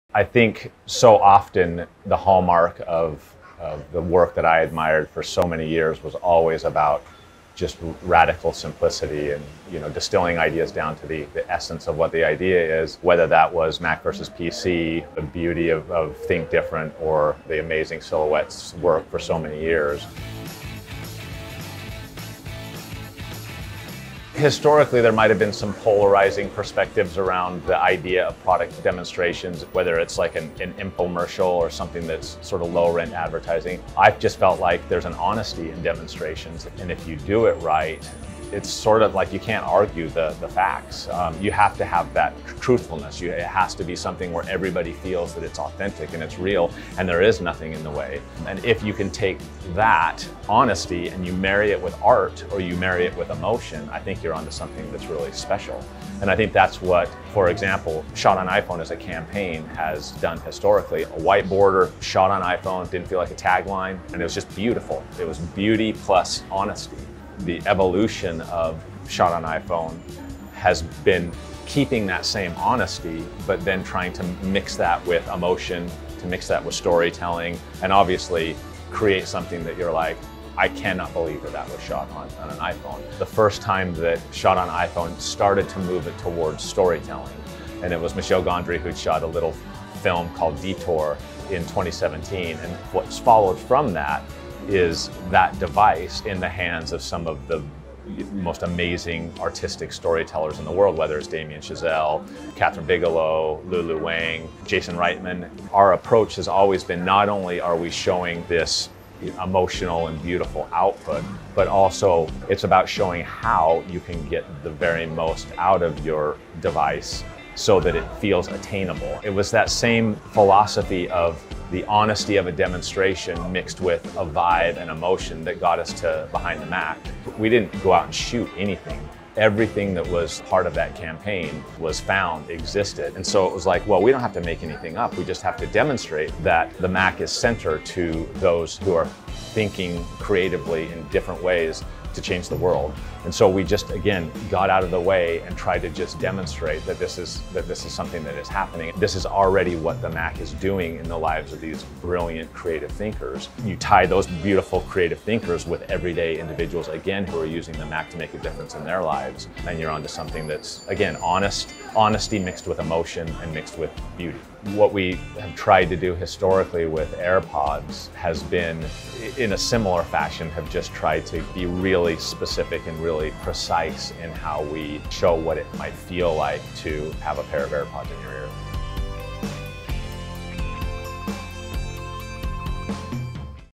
How Apple Elevated Product Demos Into Artvertising
From the jarring dystopian vibes of “1984” to the frenetic dance moves of “Silhouettes,” Apple’s ad campaigns became famous for their unmatched sense of style. Every creative marketer, including many who work for the brand now, envied Apple’s ability to sell feelings of individualism and expression instead of just selling products.

But something changed in 2014. That’s the year Apple launched “Shot on iPhone,” a campaign rooted in a minimalistic approach to one of advertising’s oldest—and often maligned—approaches: the product demonstration.

Brent Anderson, global chief creative officer of longtime Apple agency TBWA\Media Arts Lab, sat down with Adweek at the Cannes Lions International Festival of Creativity to discuss how product demonstrations became a deeply interwoven part of Apple’s creative marketing.